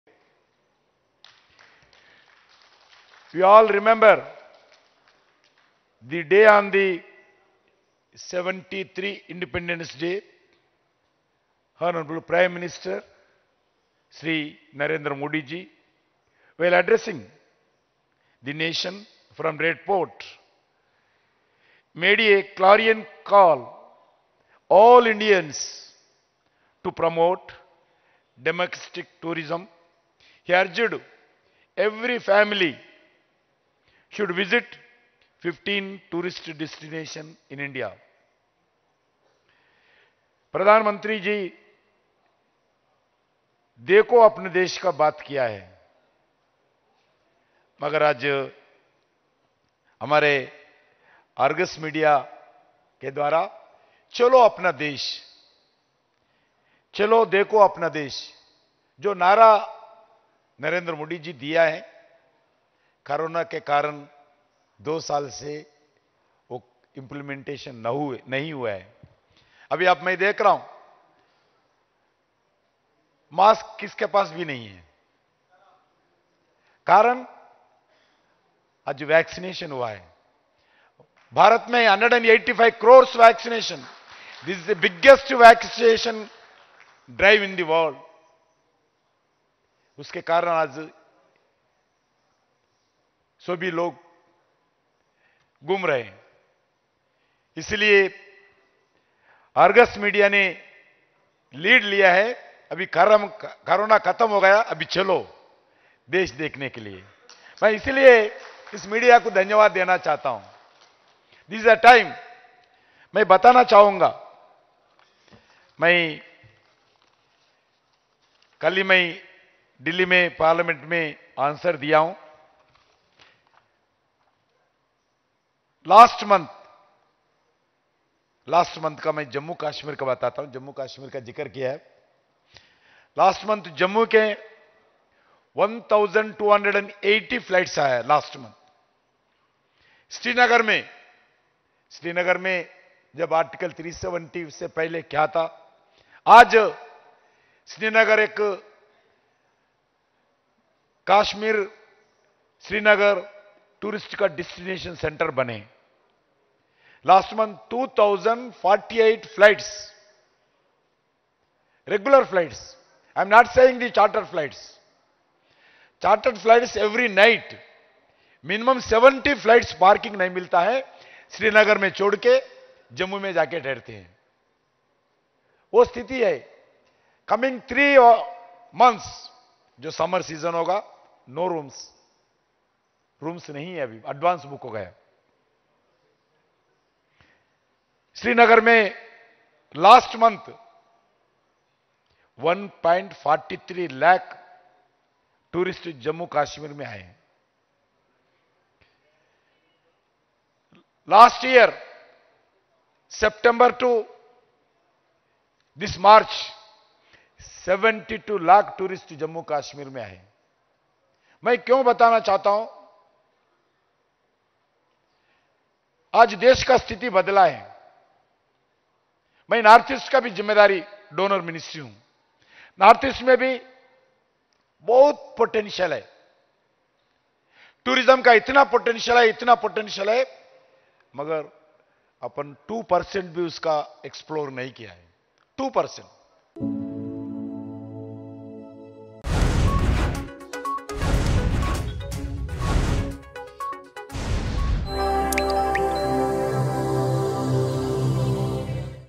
କୋଭିଡ ଭୟ କମିଛି, ବଢିଛି ପର୍ଯ୍ୟଟନ
କୋଭିଡ ଭୟ କମିଛି, ବଢିଛି ପର୍ଯ୍ୟଟନ। ପ୍ରଧାନମନ୍ତ୍ରୀଙ୍କ ନାରା ଥିଲା 'ଦେଖୋ ଆପନା ଦେଶ' ଅର୍ଗସ ନ୍ୟୁଜର କନକ୍ଲେଭ୍ ଦ୍ବାରା 'ଚଲୋ ଦେଖେ ଆପନା ଦେଶ': ଜି.କିଶାନ ରେଡ୍ଡି, କେନ୍ଦ୍ର ପର୍ଯ୍ୟଟନ ମନ୍ତ୍ରୀ